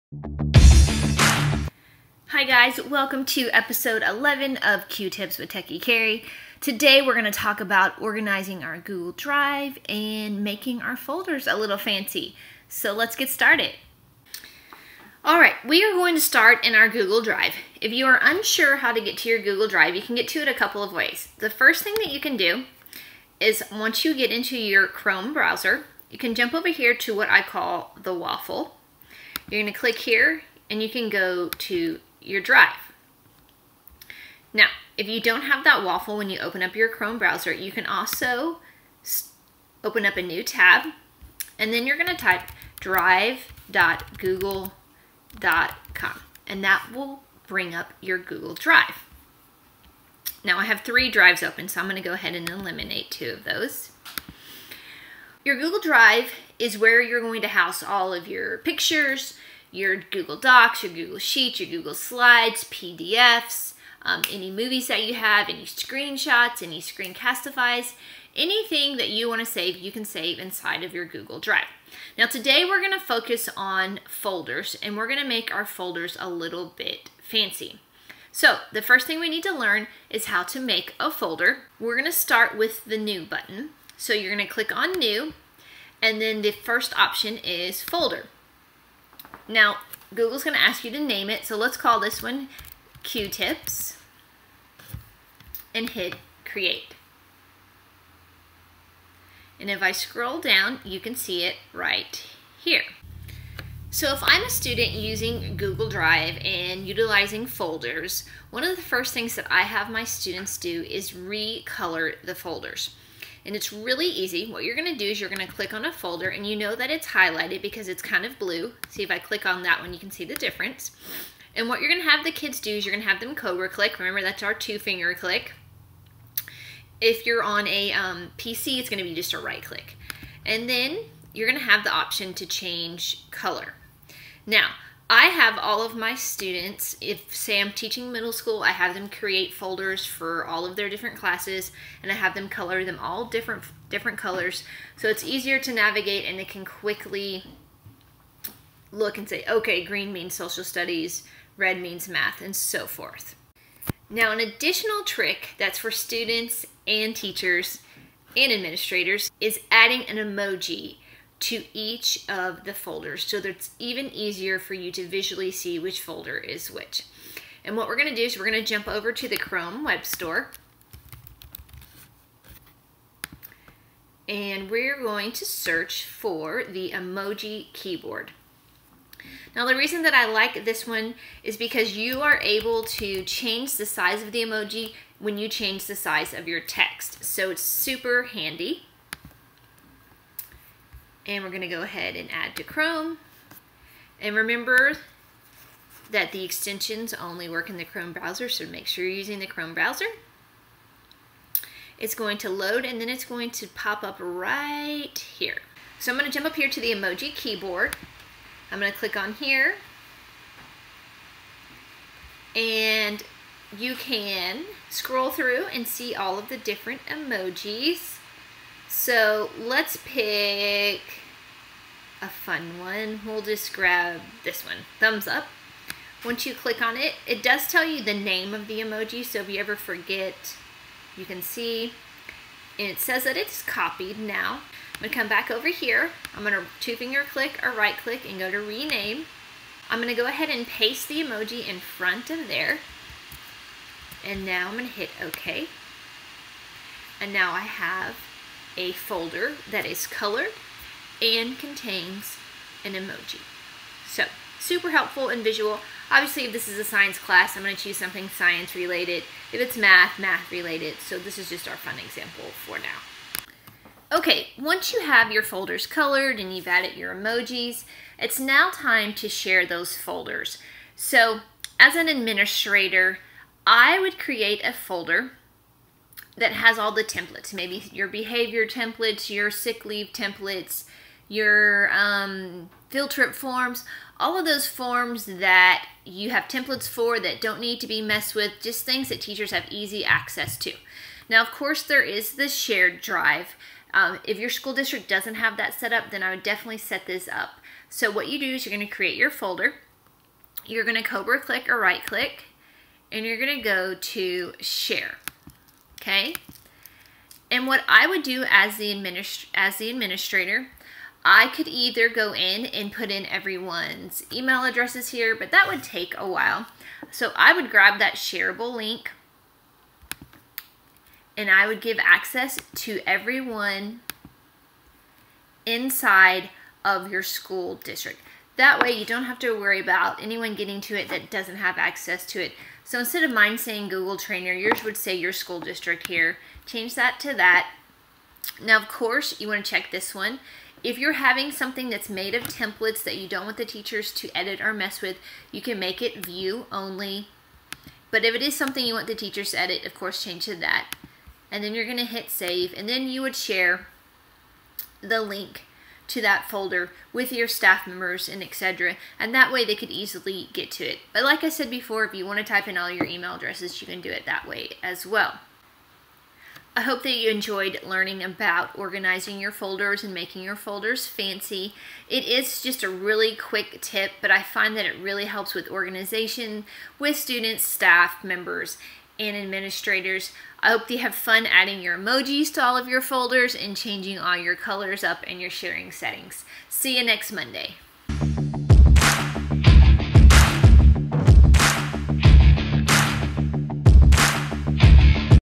Q-Tips with Techie Cari #11
Quick Tips for using Google Apps! Learn quick and easy ways to ORGANIZE and CUSTOMIZE your folders in your Google Drive! My tips are here to help students, teachers, and administrators!